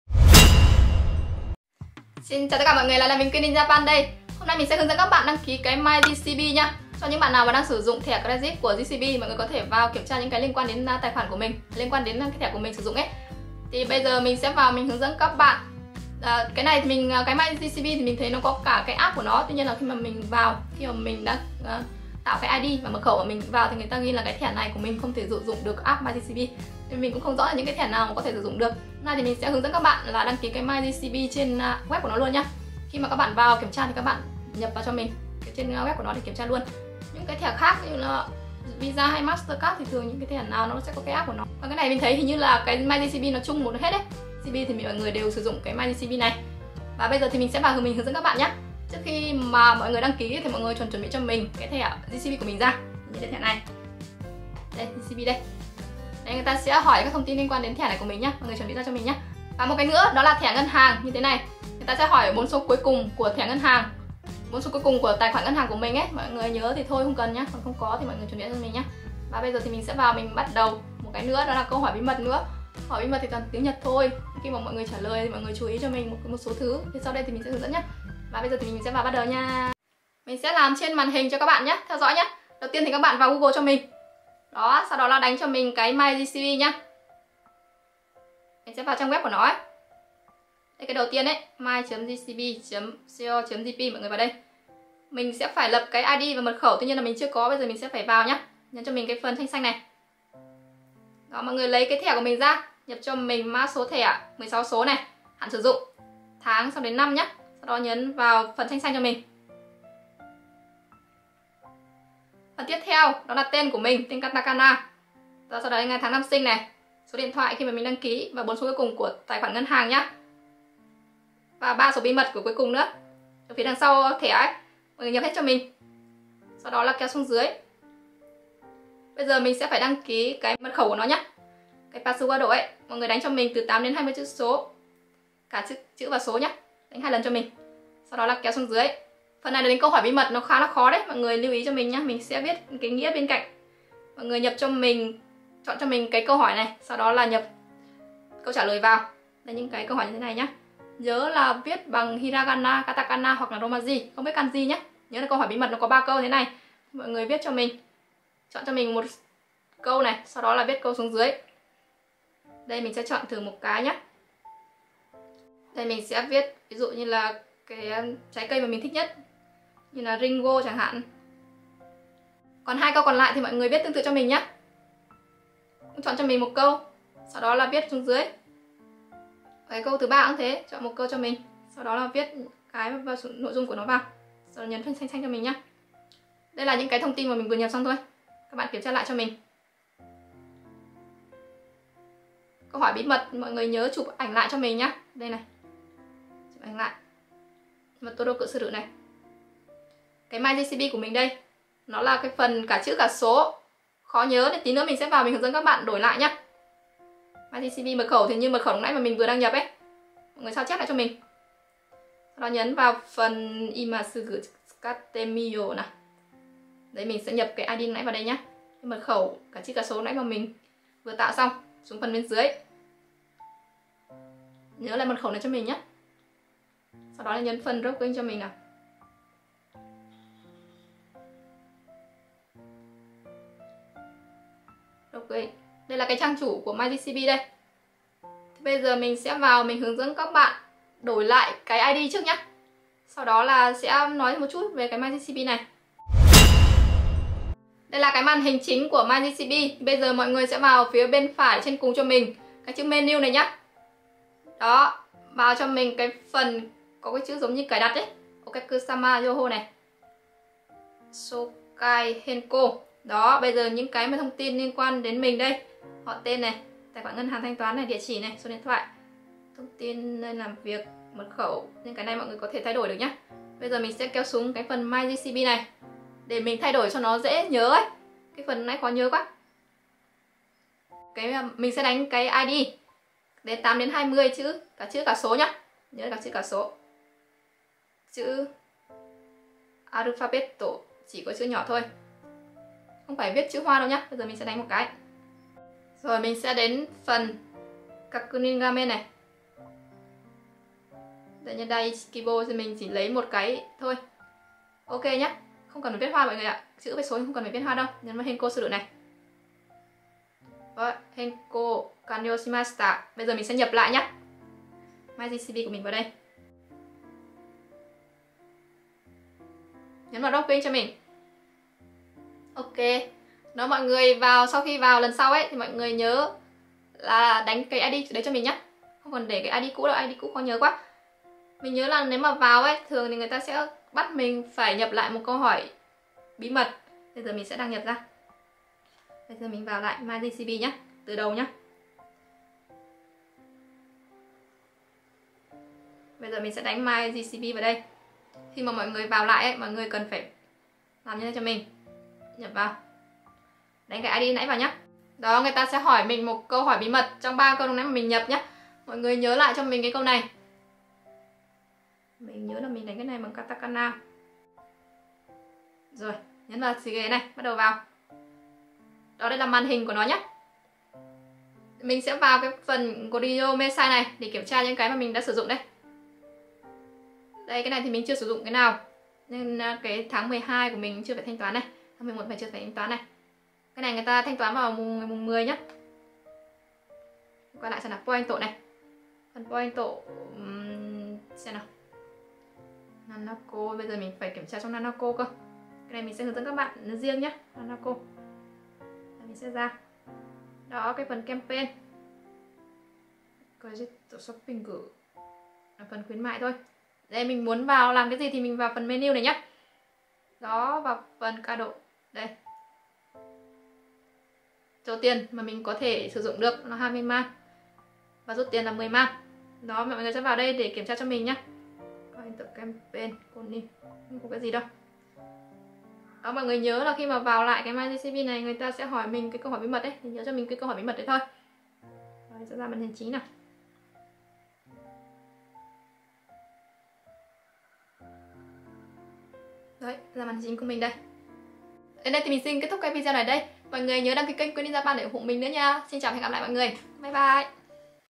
Hướng Dẫn Chi Tiết Cách Cài Đặt App My JCB Thẻ Có Ghi Mã Số -Quyen in JAPAN✅
Cám ơn các bạn luôn theo dõi kênh Quyen in JAPAN!
Trang chủ My JCB:
My jcb là trang chung cho các thẻ của JCB nên các bạn sử dụng thẻ credit JCB nào cũng có thể đăng nhập vào đây để kiểm tra thông tin thẻ của mình.
Cám ơn sự theo dõi của các bạn!